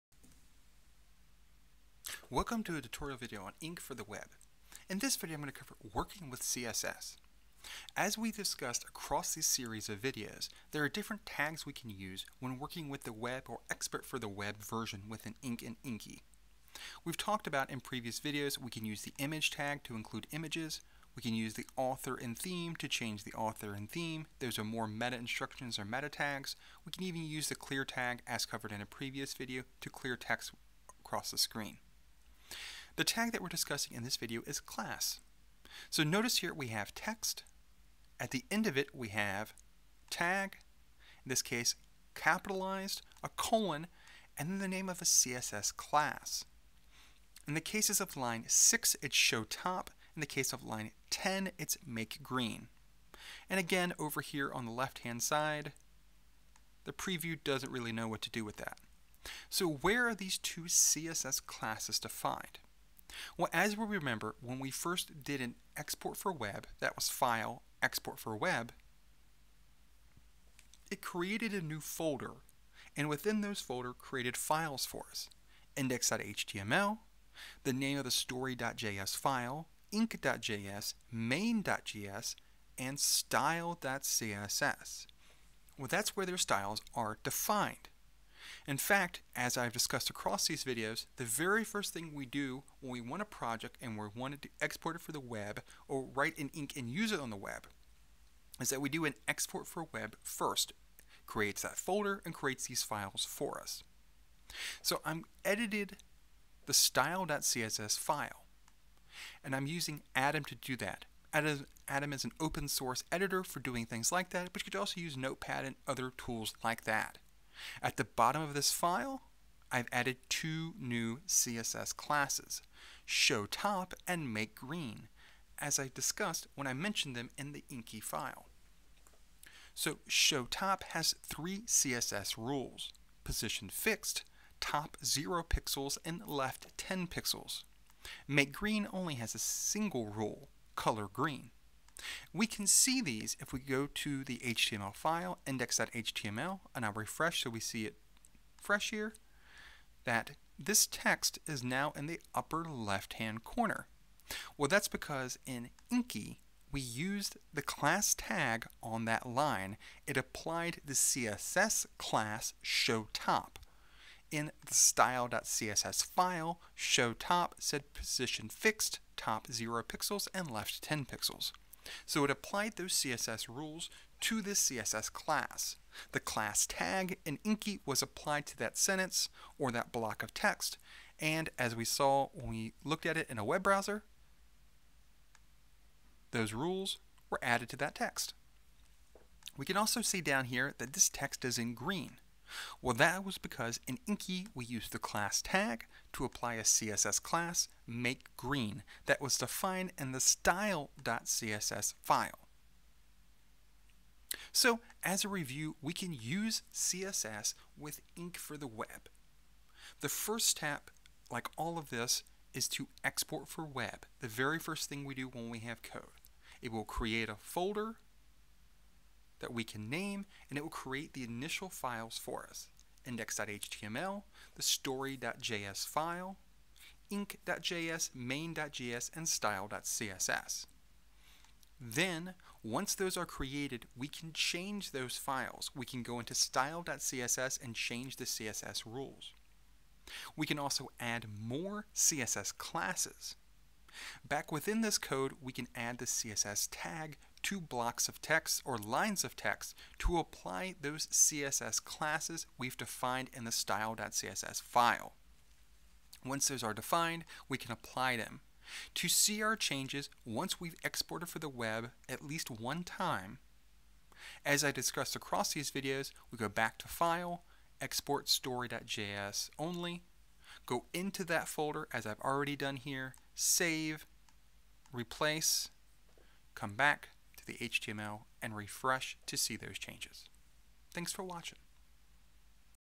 Ink for the Web Part 4 Working with CSS (Inky 0.10.0 / Ink 0.8.2)
This video reviews how to work with CSS and the CLASS tag within the "style.css" file created during the "Export for web..." process.